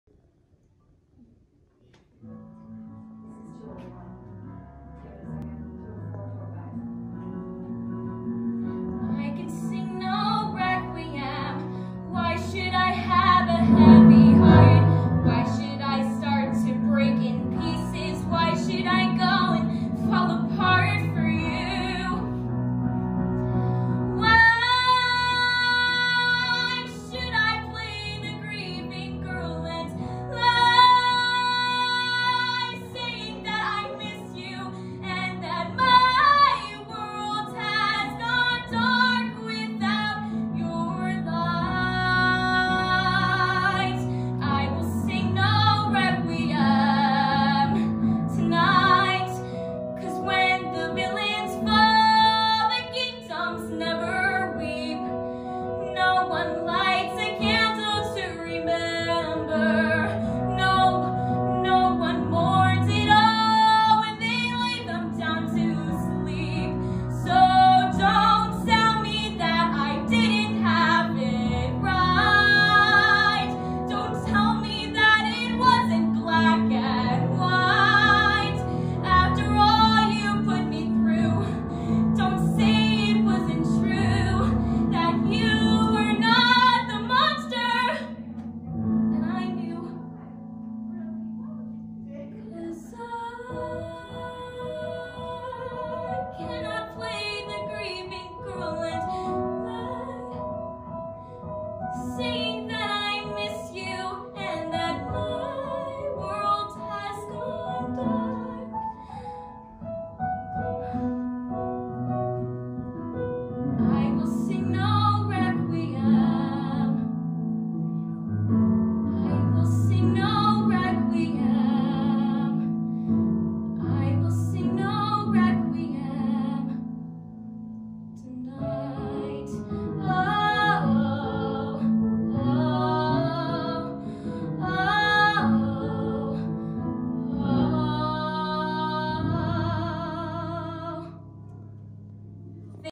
Model Act Studios Talent Ava
Watch Model Act Studios Talent Avas Performance at Model Act Studios Schaumburg Studio!!!